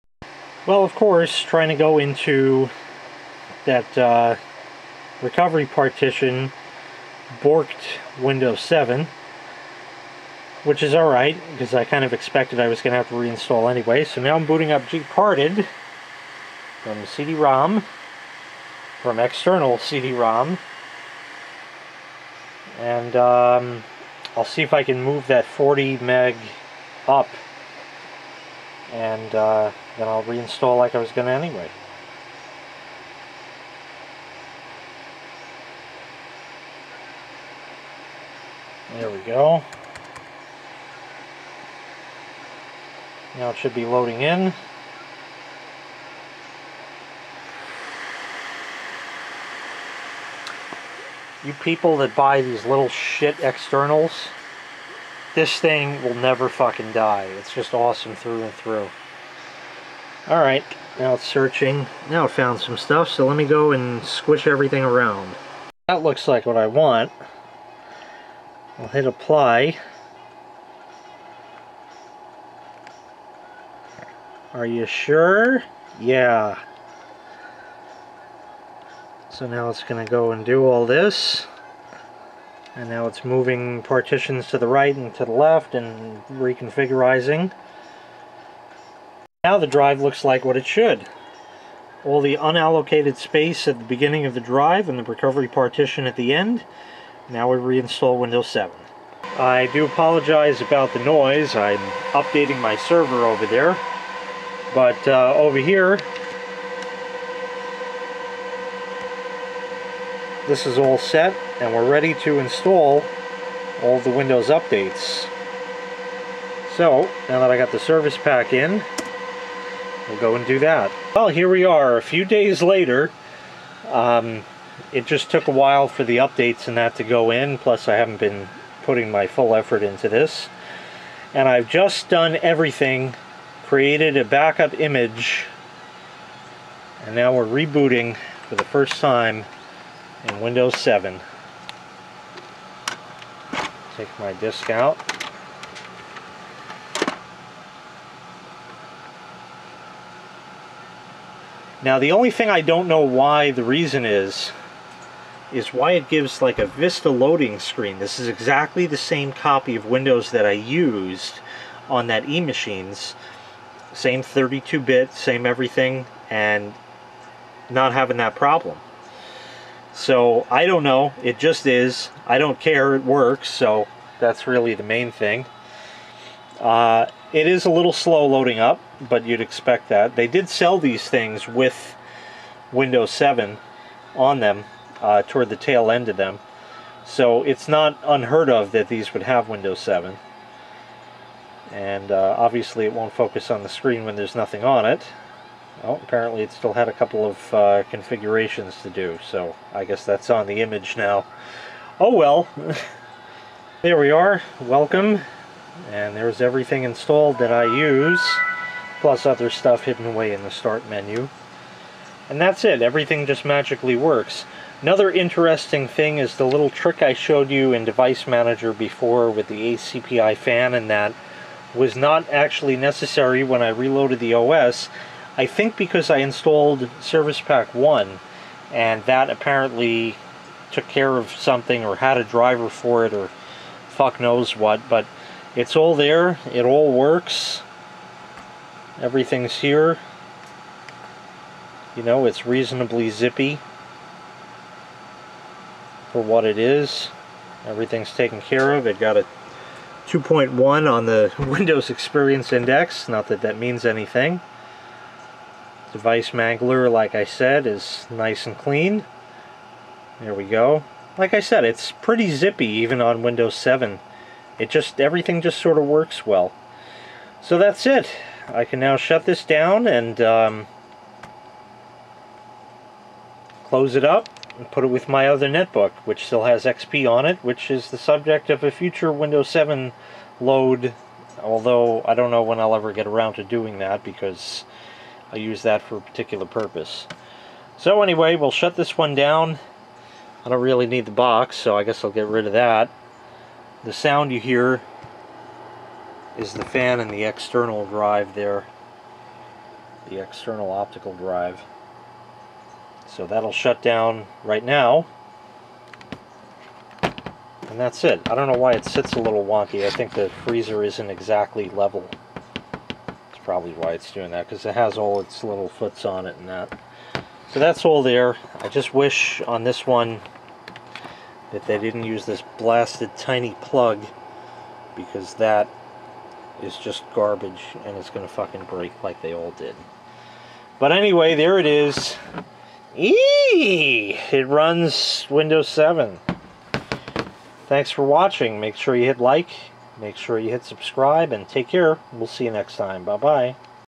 ASUS Eee PC Refurbishment, Part 3 - Moving Recovery Partition, Finished Windows 7 Install
Originally recorded March 13, 2017.

Of course, going into the recovery partition borked Windows 7.  So I ran GPartEd and reconfigured the partitions properly.

Took an image afterwards, everything is up and running.  Or is it...

Some more will follow in this series.